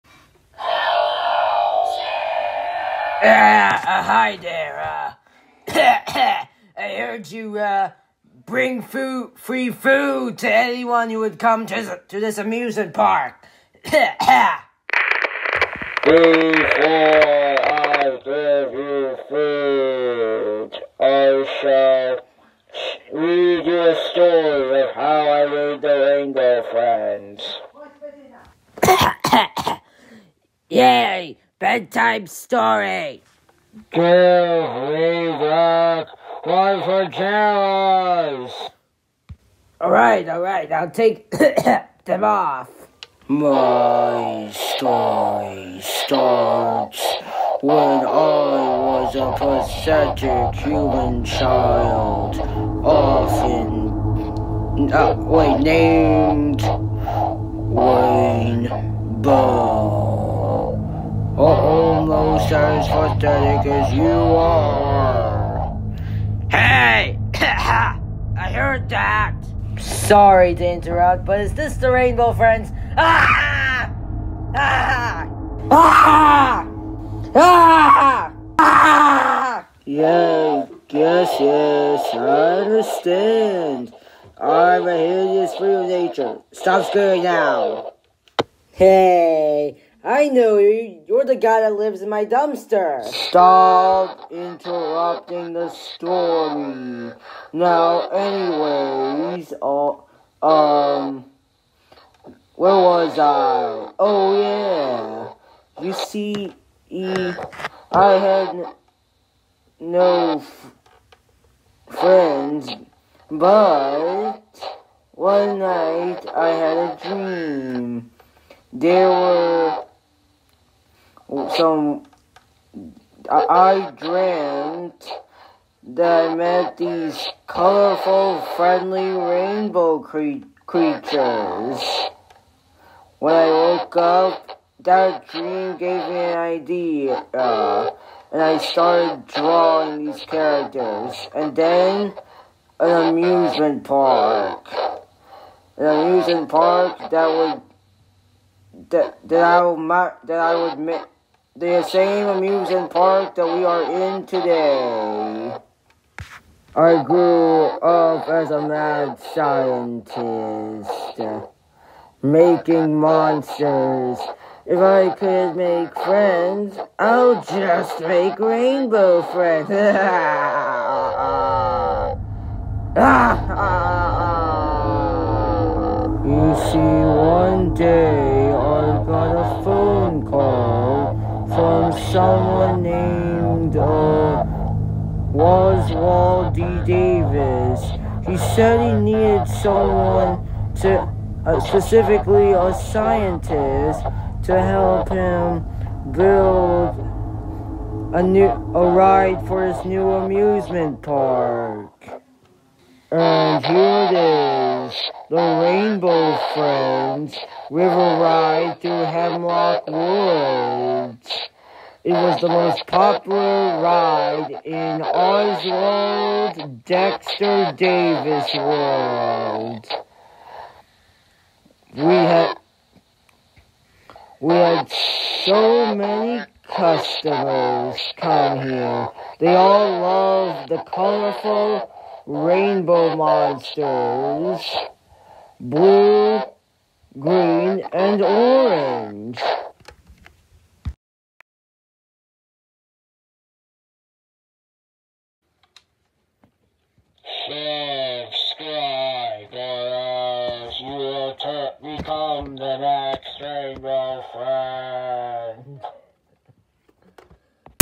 Rainbow Friends Episode 1: The Backstory
The scientist, Red, explains the backstory of how he turned himself and everyone else into Rainbow Friends.